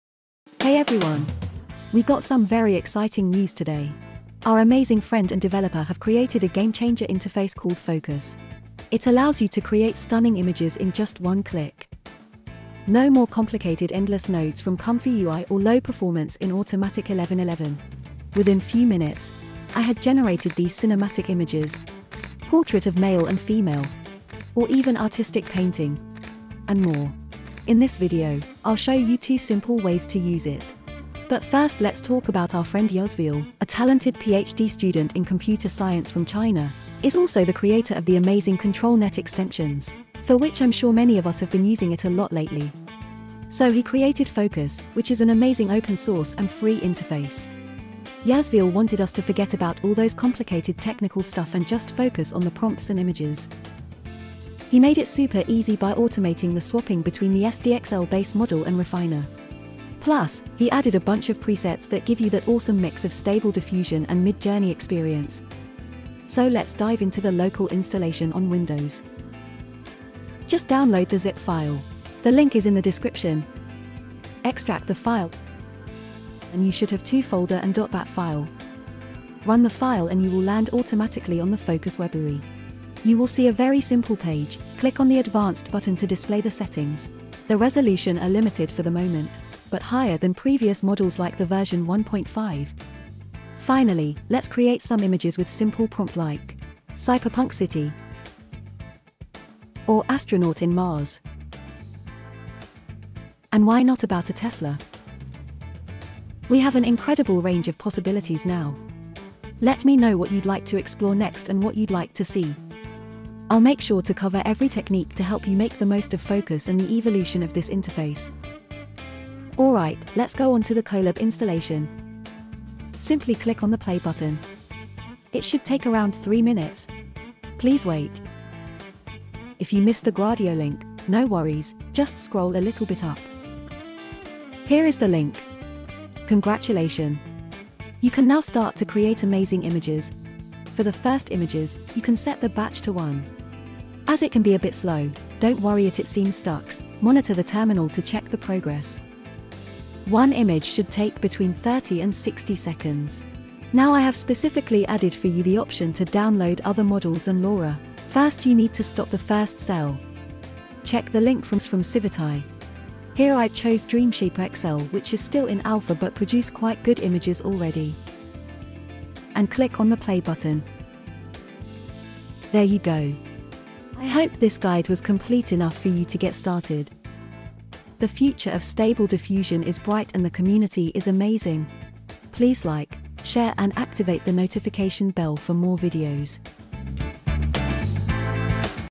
Use SDXL like Midjourney - Fooocus | Google Colab | No ComfyUI Nodes or A1111 | Local Tutorial
Introducing SDXL with Fooocus UI by IIIyasviel himself ! He's also the creator of controlnet.

This UI doesn't need any nodes and is performing as well as A1111. With Fooocus, you can create stunning visuals with just one click!

I build two convenient methods of use : locally and on google colab. You can either opt for a local installation or utilize the online version with Colab (no GPU required).

This is really exciting for the stable diffusion community !